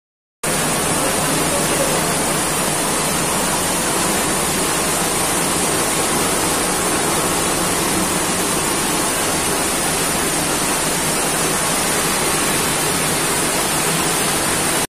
Cooling Tower Water Circulation | Mechanical Engineering Skills
This video is to help you understand how does the cooling tower water circulation.